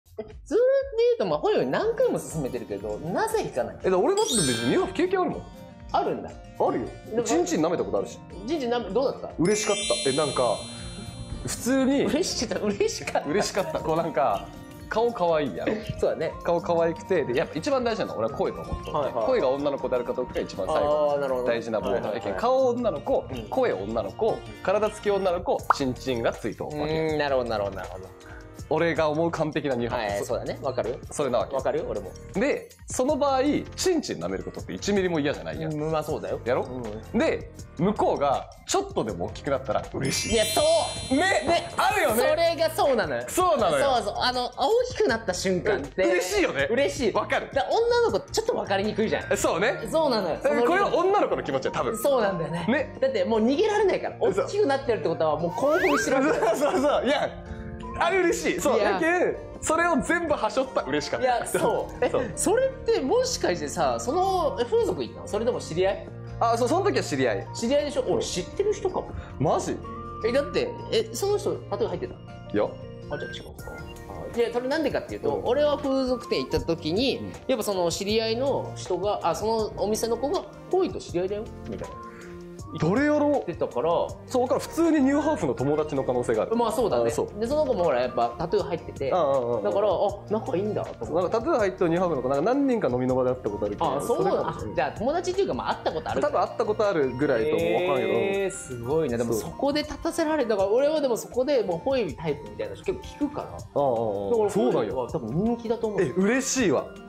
男の娘は最高！！経験者DJふぉいが男の娘の良さを語る【DJふぉい＆DJ脇】【切り抜き】
DJふぉいさんの切り抜き動画を投稿しています。